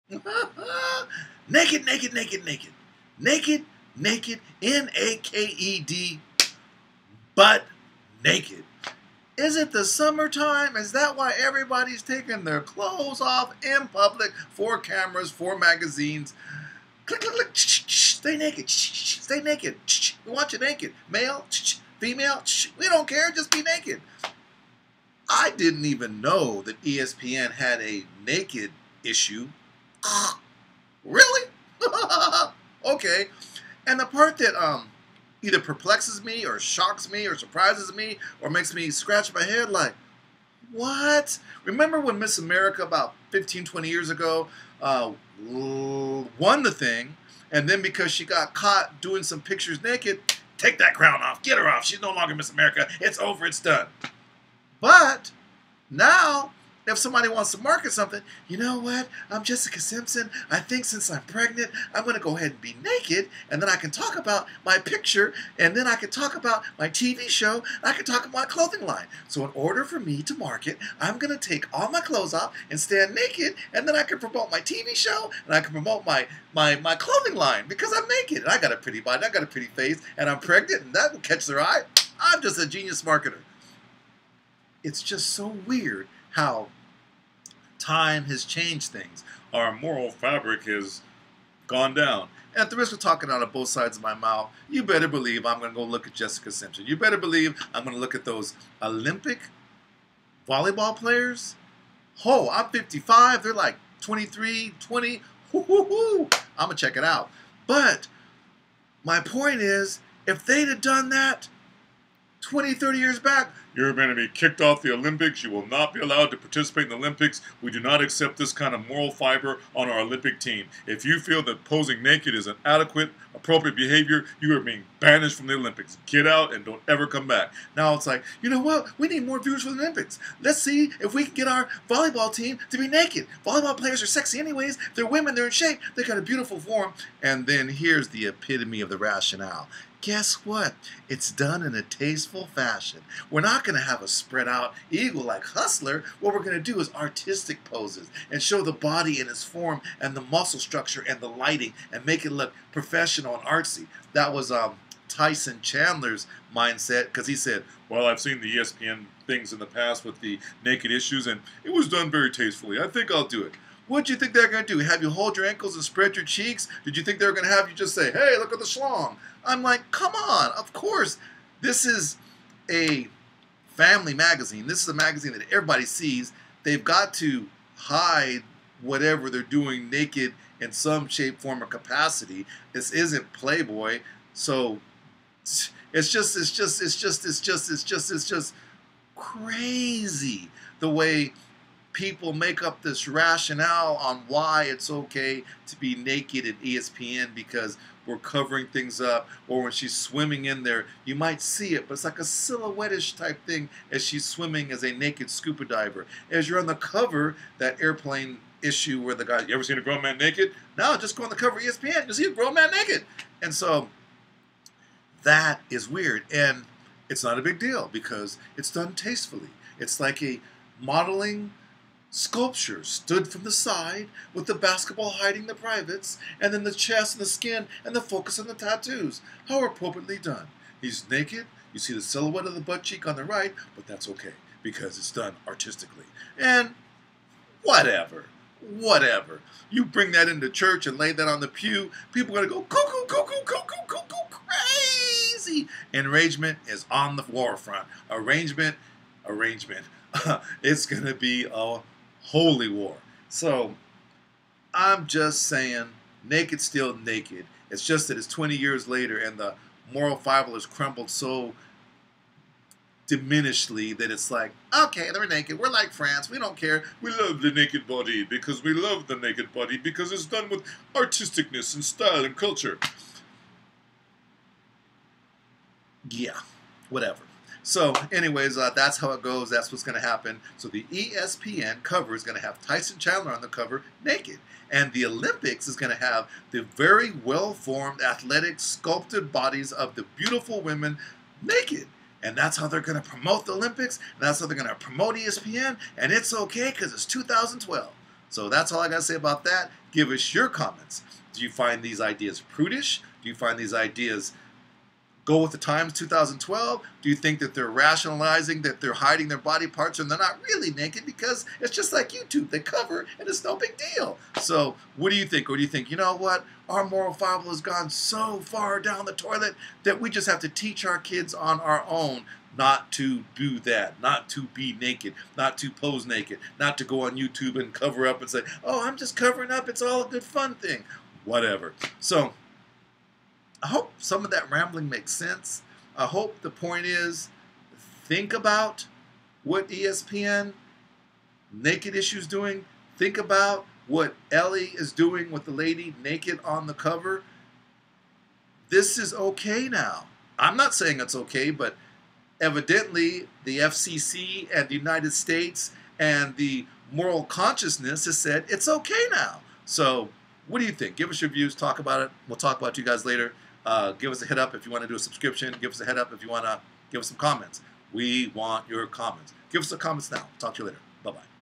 woman volleyball players pose naked for sports magazine
Chandler is one of 27 athletes who posed nude for the issue. There are four different covers, but the one featuring Chandler is the first to be released,